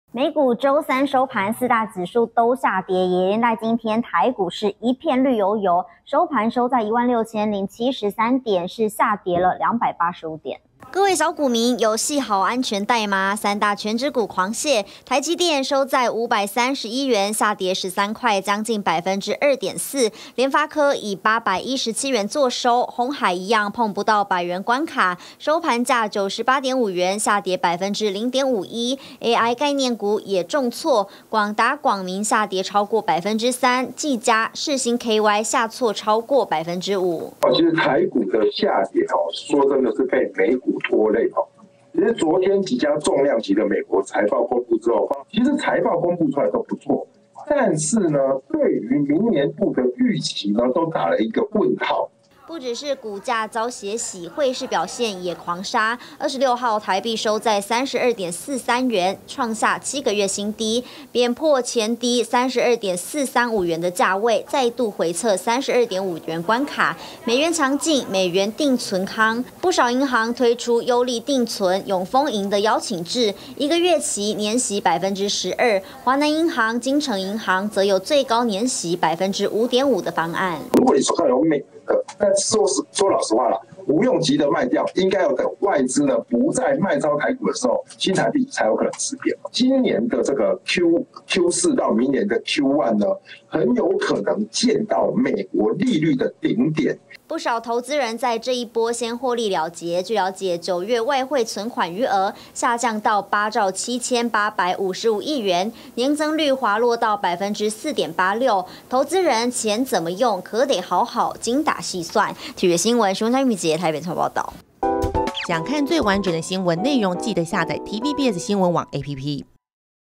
雙殺! 台股跌285點 台幣收32.43元 7個月新低｜TVBS新聞
美股四大指數重挫，連帶台股跳空，下跌285點，收在16703點，台積電下跌超過2%，前陣子話題性十足的AI概念股，也慘遭血洗，股匯雙殺，台幣今天以32.43元作收，美元強勁不少投資人已經先賣掉獲利了結，專家建議還可以繼續存，大約今年第四季到明年第一季之間，能看見美元頂點。